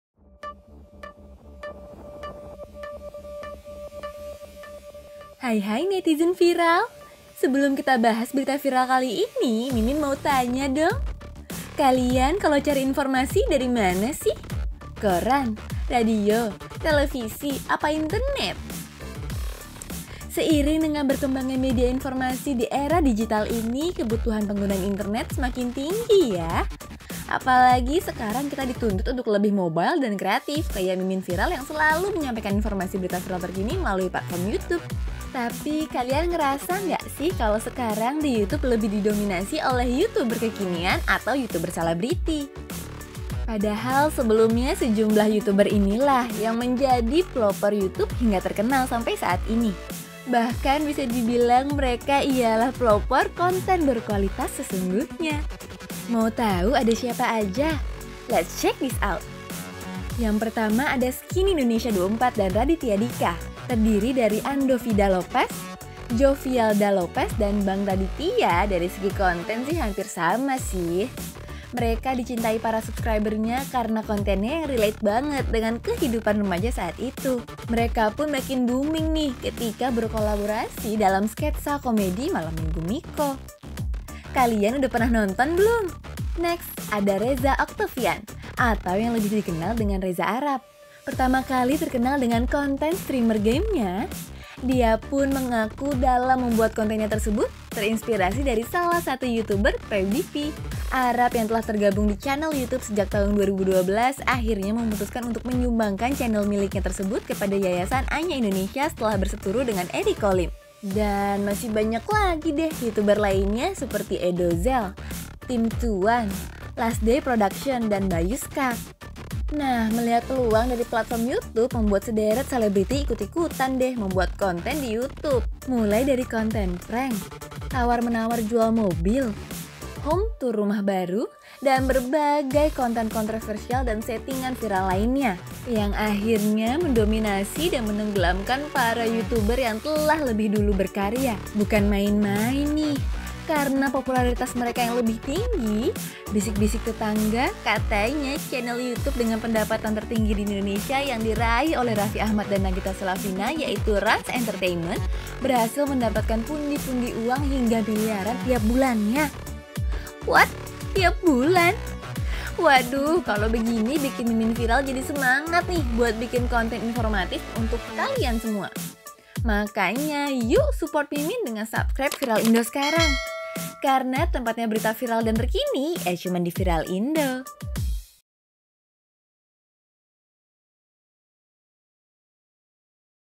VIRAL! YOUTUBER INDONESIA MULAI KEHILANGAN JATI DIRI, SEMENJAK....
Sobat Viral!
Kalian masih inget gak pas zamam Youtube ketika belum dijamah sama para artis?
Konten yang disajikan menurut mimin jauh lebih berkualitas daripada sekarang.
Gimana menurut sobat?

Kali ini Tim Viral akan bahas perkembangan Youtube yang nampaknya udah berubah nih.
Penasaran? Simak videonya sampai habis!

Nyalakan juga loncengnya biar keep uptodate video Viral selanjutnya!

Viral Indo!
Berita Viral Terkini ya cuma disini!